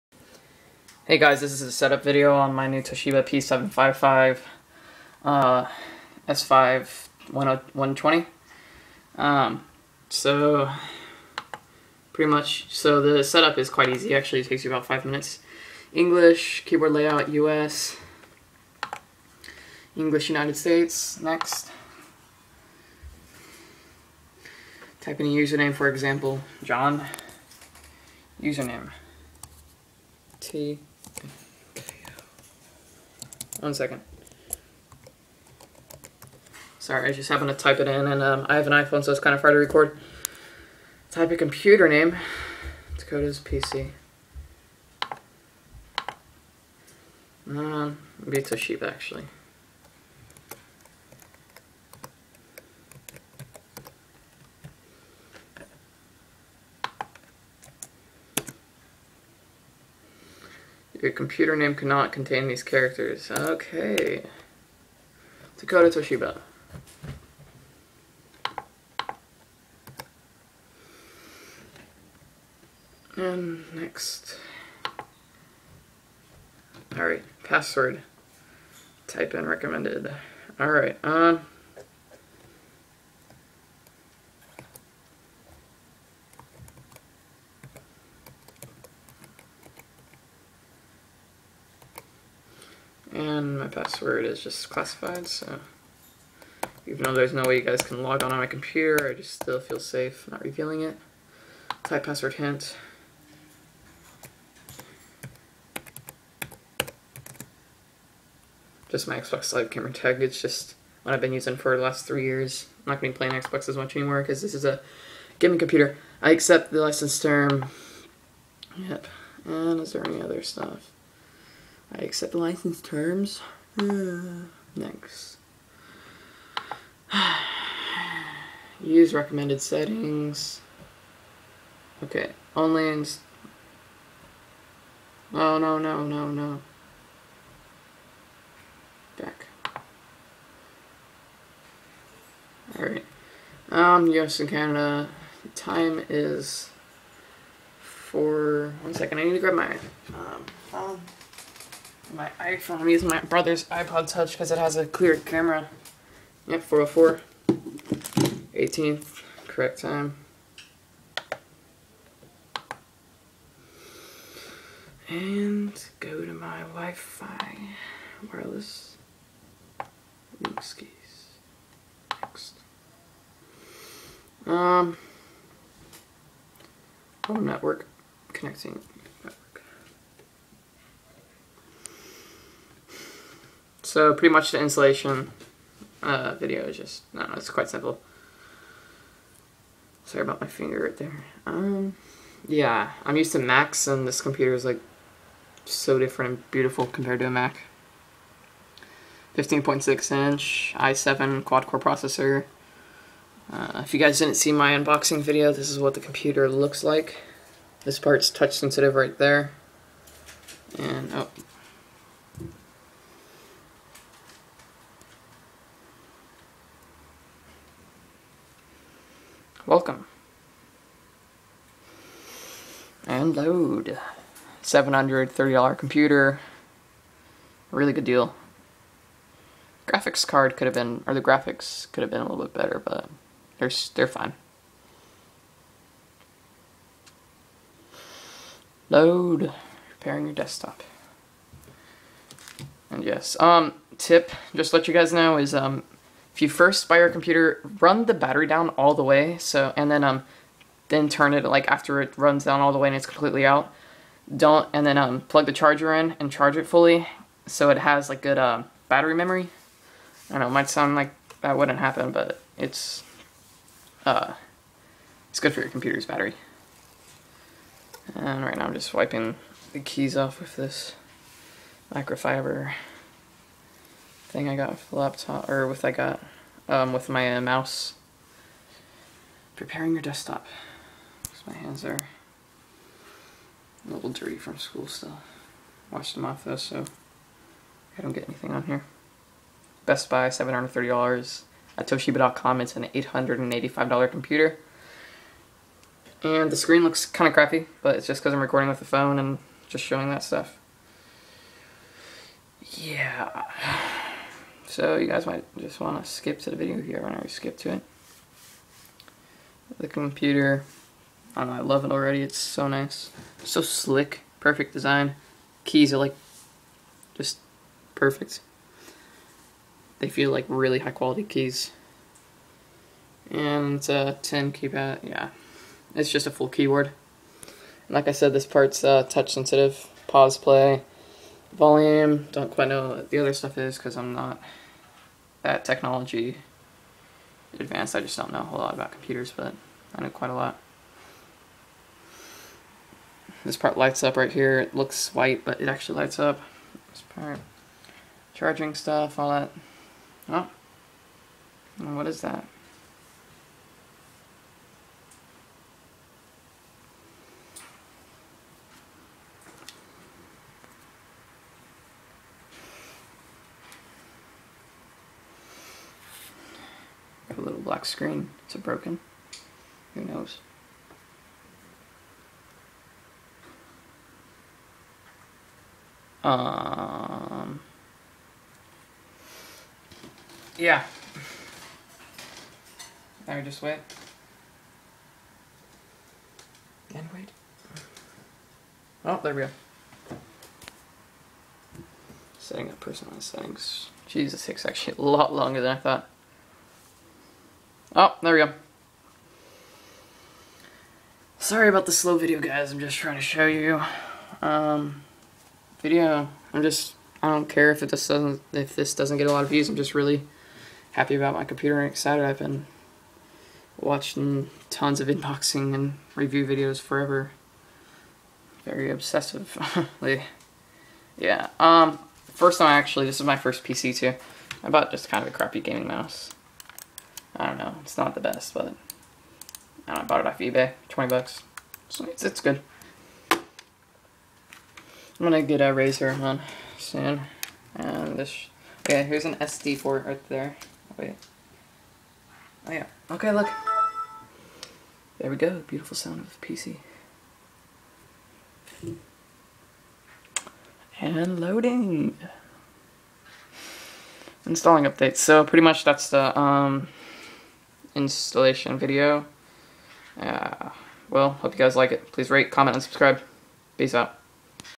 Toshiba sateillite Set Up guide
This is my set up video on how to set up a Toshiba/Windows 7 laptop.